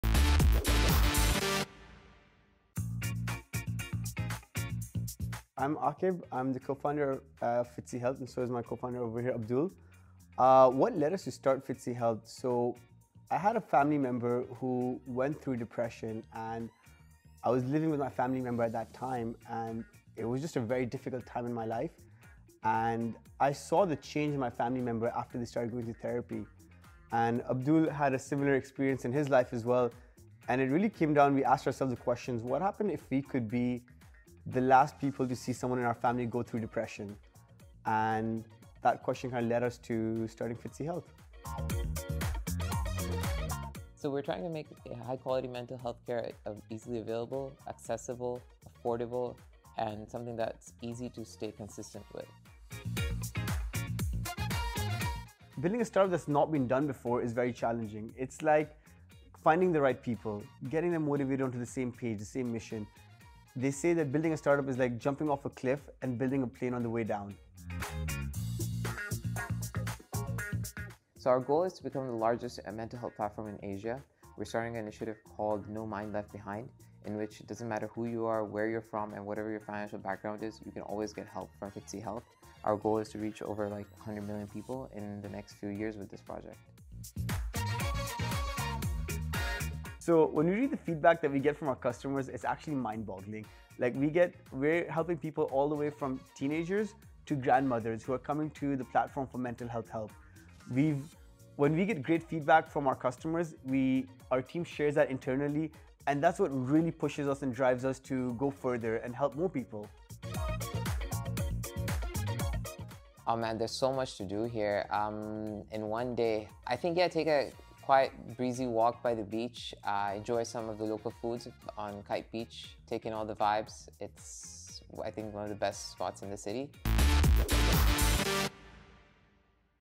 DXB Today Spotlight: Fitcy Health hopes to provide affordable therapy for all
Fitcy Health hopes to provide affordable therapy for all

Fitcy Health is on a mission to provide affordable therapy for all. Learn about their services and how they're making mental health care more accessible